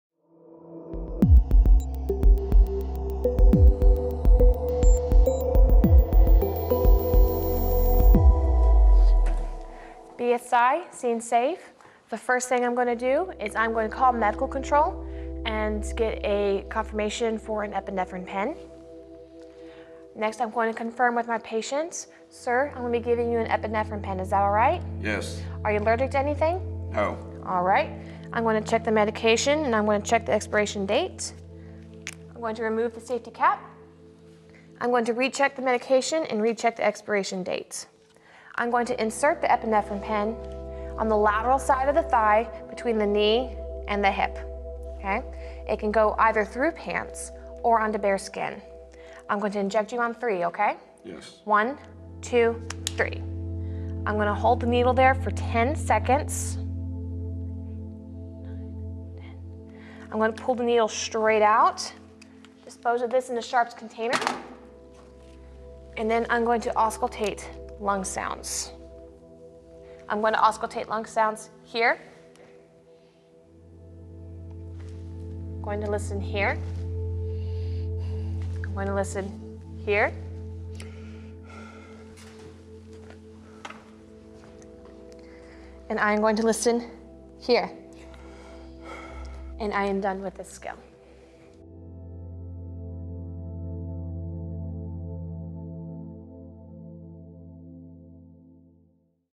EPI PEN AUTO INJECTOR RC Health Services
National Registry Skills Video
Epi Pen Auto Injector
EMT Basic Training
RC Health Services EMS Program